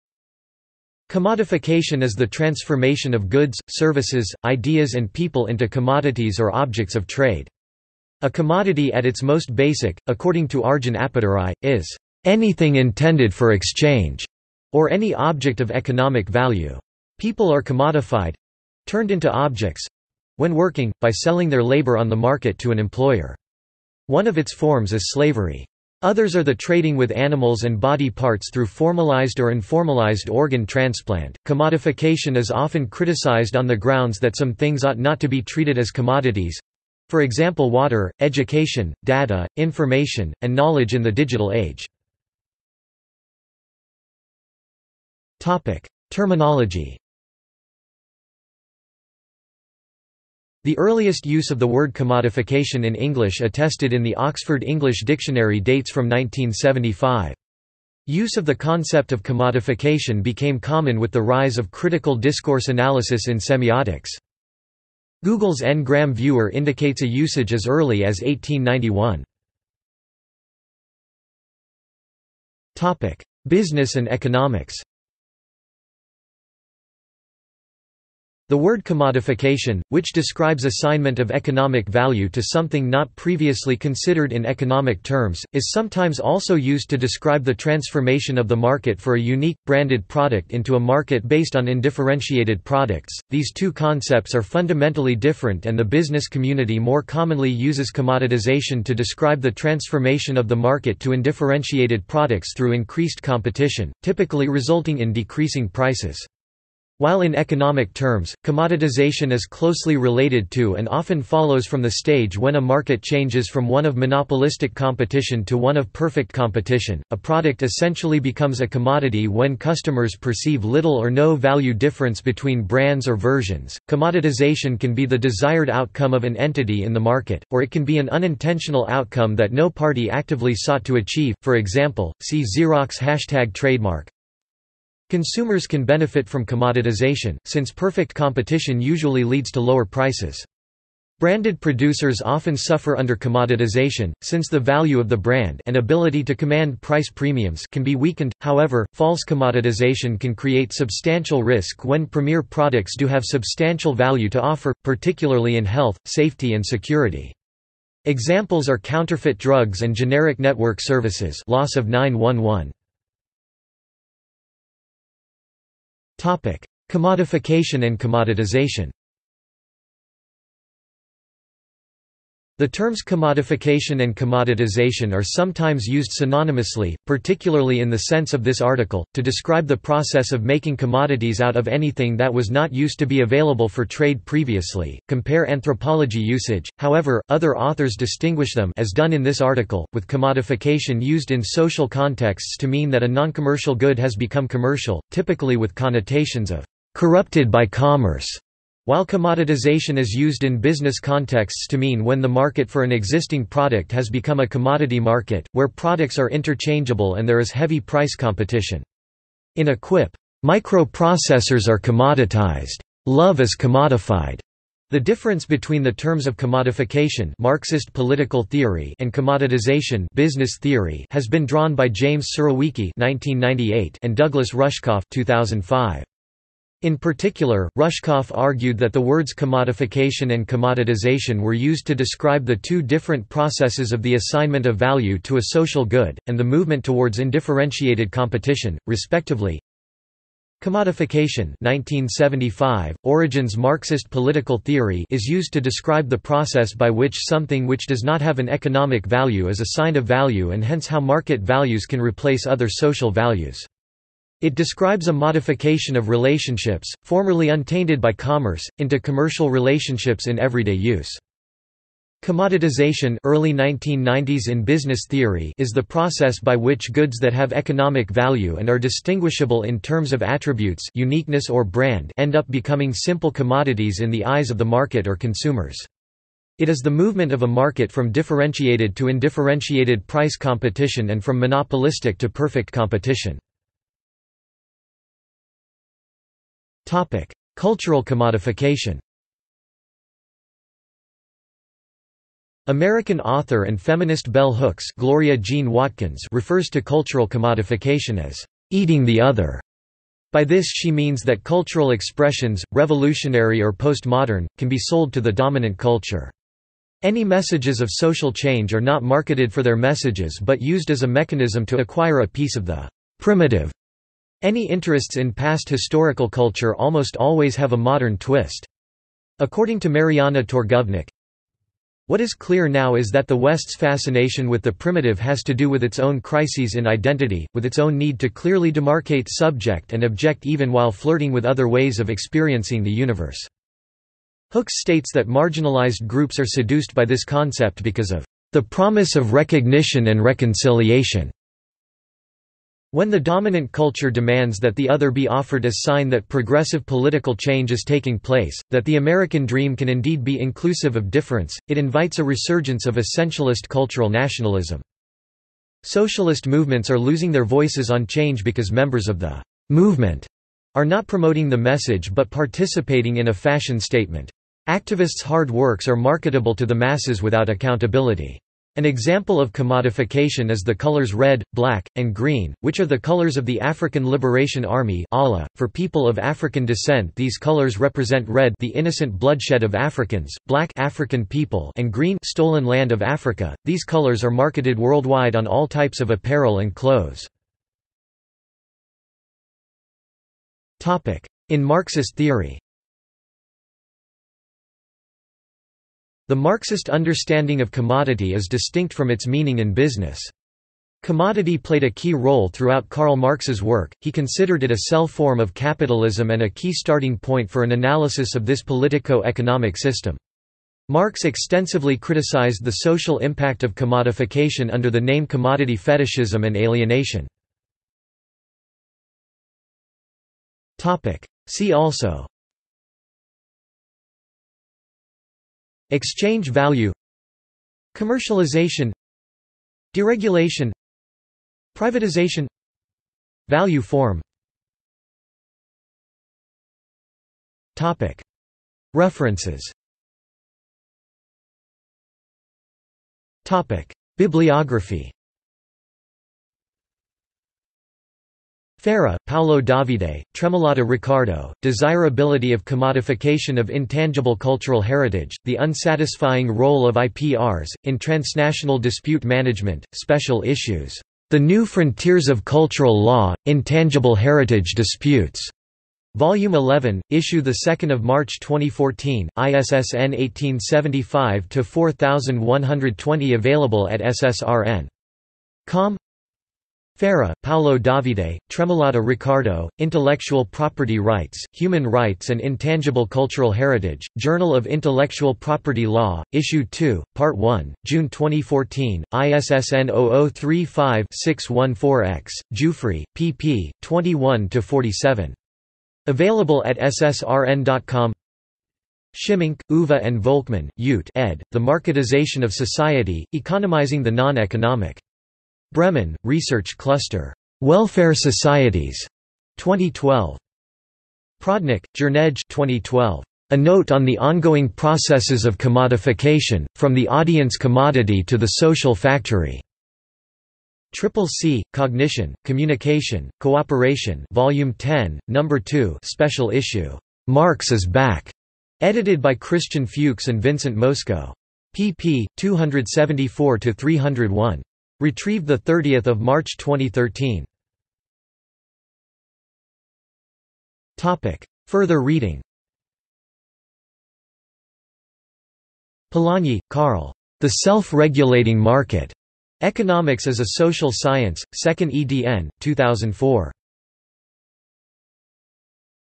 Commodification | Wikipedia audio article
This is an audio version of the Wikipedia Article:
Commodification

- Socrates

SUMMARY
Commodification is the transformation of goods, services, ideas and people into commodities or objects of trade. A commodity at its most basic, according to Arjun Appadurai, is "anything intended for exchange," or any object of economic value. People are commodified—turned into objects—when working, by selling their labour on the market to an employer. One of its forms is slavery. Others are the trading with animals and body parts through formalised or informalised organ transplant.Commodification is often criticised on the grounds that some things ought not to be treated as commodities—for example water, education, data, information, and knowledge in the digital age.